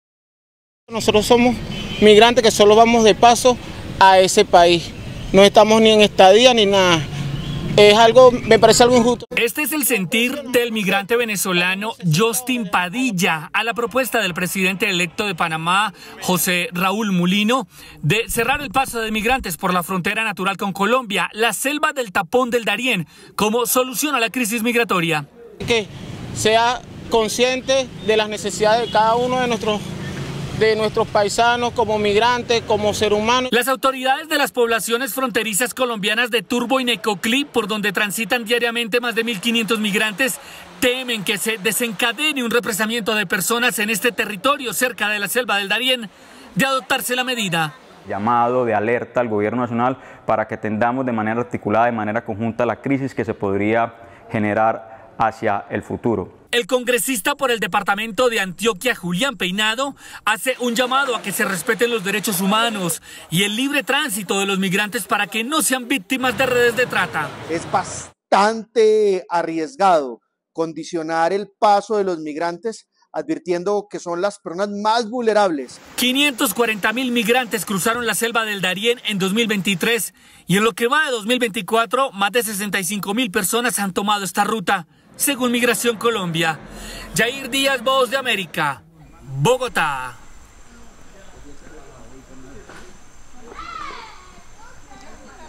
Propuesta de cerrar paso por el Darién provoca temor en Colombia
Como una solución a la crisis migratoria, el presidente electo de Panamá José Raúl Mulino ha propuesto cerrar el paso por la selva del Darién, en la frontera con Colombia. Jair Díaz de la Voz de América, nos cuenta que del lado colombiano temen una represión de personas, de adoptarse esta medida.